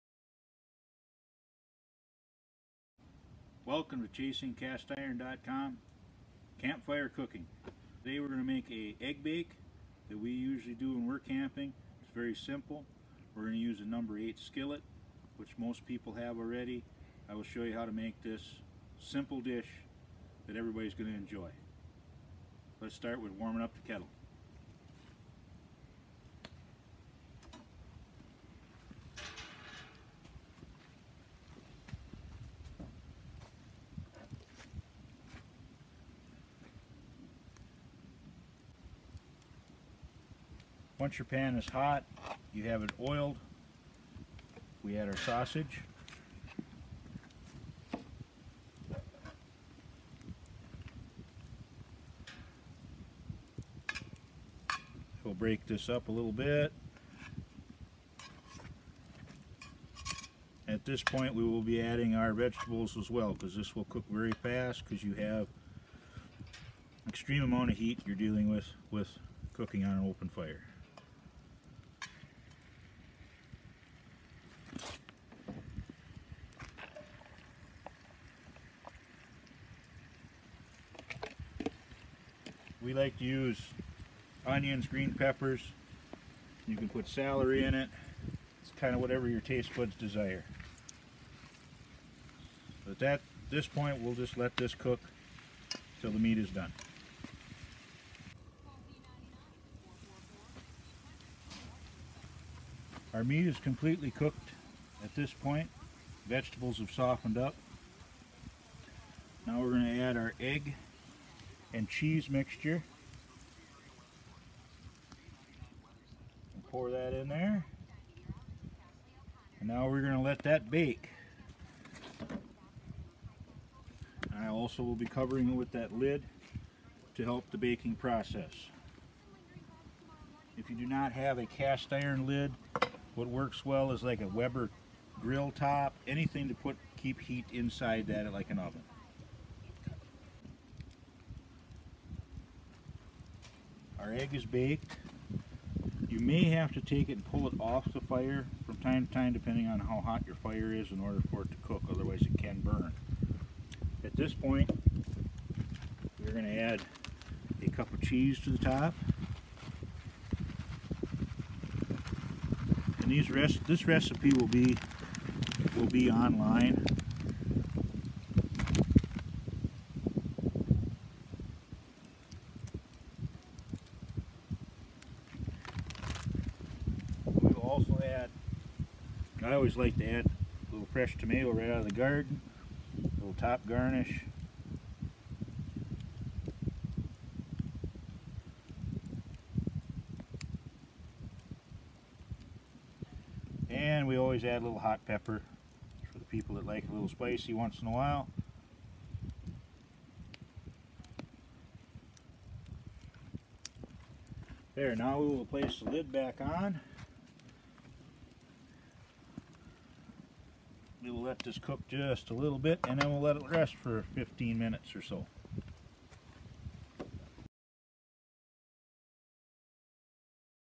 Egg Bake - Campfire
Egg bake baked in a cast iron skillet over a campfire.